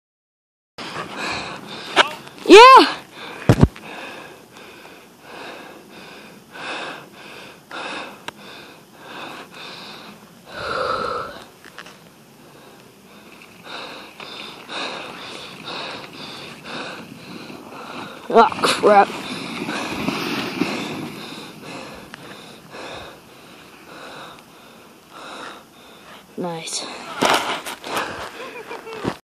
My turn
Title says it all.  Toby wouldn't take his hands out of his gloves so he started the video with his nose.  LOL